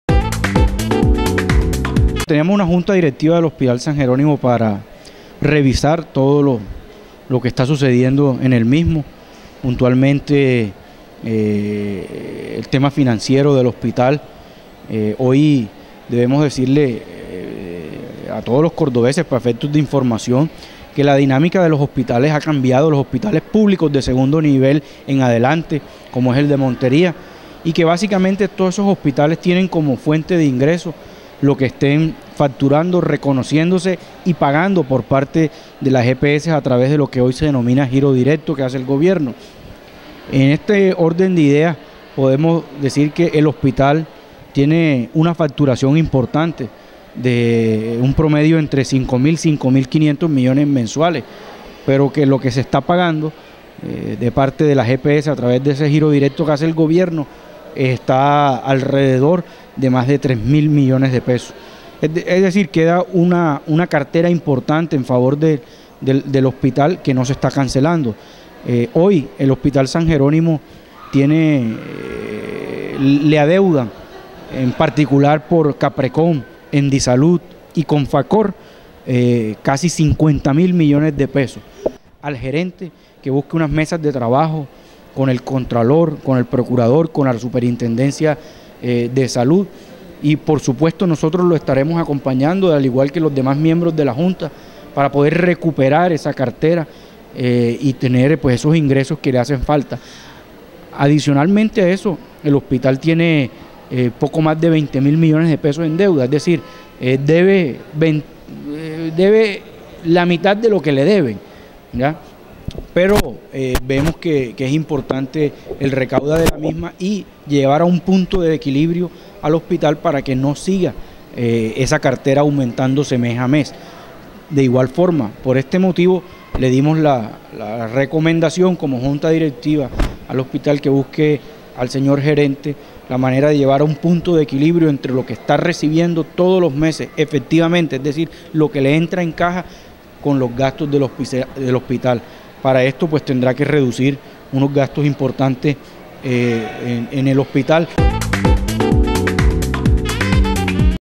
GOBERNADOR SOLICITÓ A LA GERENCIA DEL HOSPITAL DE MONTERÍA UN PLAN DE CHOQUE
GOBERNADOR ALEJANDRO LYONS MUSKUS SOLICITÓ A LA GERENCIA DEL HOSPITAL SAN JERÓNIMO DE MONTERÍA PRESENTAR UN PLAN DE CHOQUE PARA RECUPERAR LA CARTERA MOROSA POR MÁS DE 50 MIL MILLONES DE PESOS QUE LE ADEUDAN LAS EPS Y PRESTAR UN SERVICIO DE SALUD CON CALIDAD